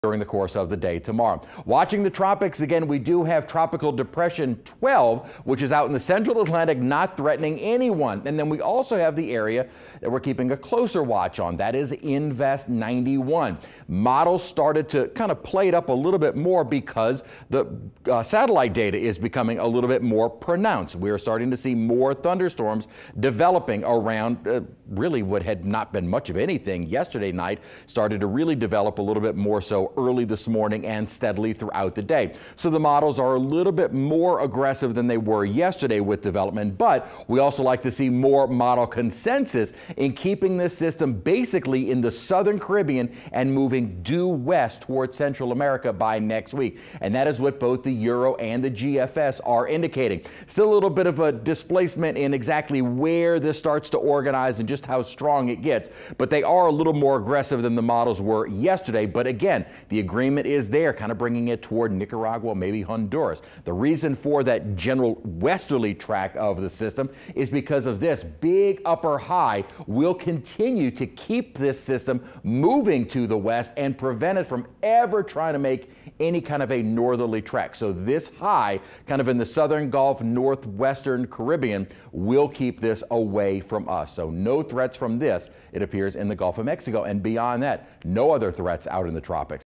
Tuesday evening tropical update: Invest 91 and TD 12
In the Tuesday evening tropical forecast, Chief Meterologist Chris Franklin says we are tracking Invest 91 and TD 12.

Invest 92 in the eastern Atlantic was recently upgraded to TD 12. Doesn't appear to threaten land even in the long-range models. The official forecast calls for the depression to strengthen to a Tropical Storm before weakening by Thursday as the surrounding environment becomes more hostile.